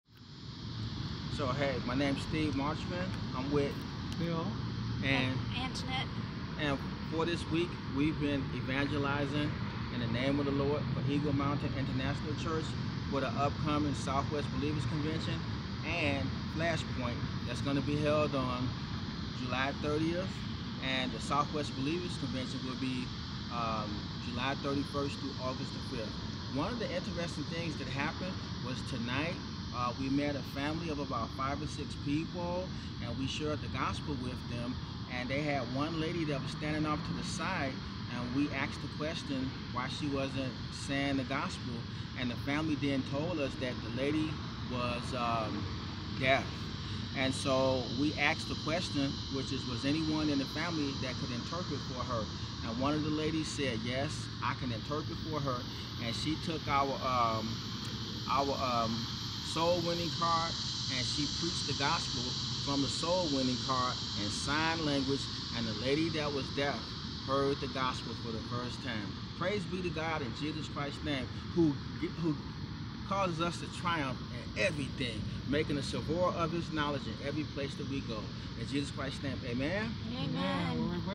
Steven's Testimony
You’ve got to check this out! A lady translated our card using sign to lead her friend to Jesus!